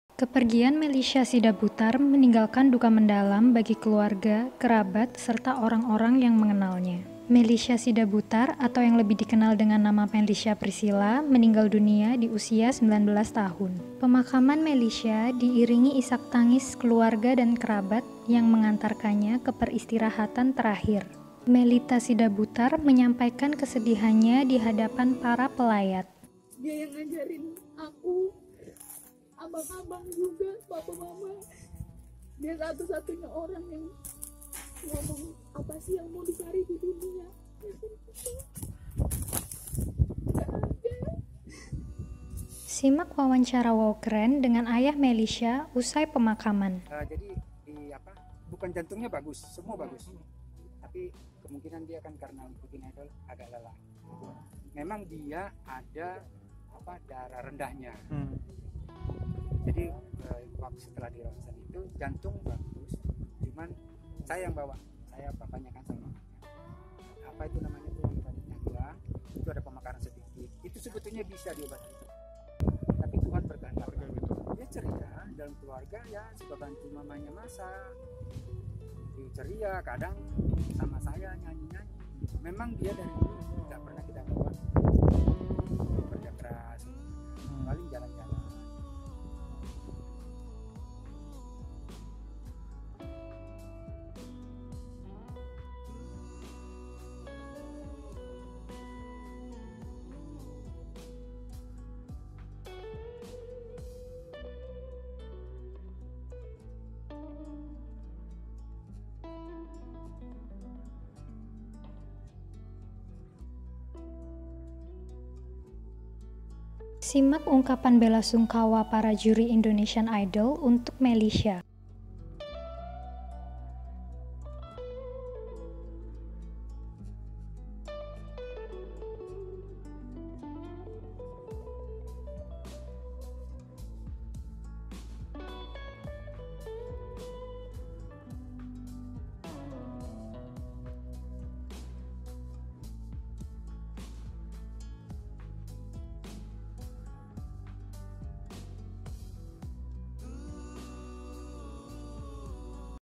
Kronologi Melisha Sidabutar Meninggal Dunia Terungkap, Begini Duka Keluarga hingga Juri 'Indonesian
Dunia hiburan Tanah Air kembali berduka. Salah satu kontestan "Indonesian Idol" Special Season atau season 11, Melisha Sidabutar, telah meninggal dunia pada Selasa (9/12). Lantas kini jasadnya pun dimakamkan di TPU Pedurenan, kawasan Mustika, Bekasi, Jawa Barat, Kamis (10/12). Proses pemakaman Melisha yang dimulai sekitar pukul 12.00 WIB berjalan lancar. Doa-doa dan lagu pujian pun mengiringi pemakaman kontestan Indonesian Idol itu. Apa kata kembaran dan ayah Melisha?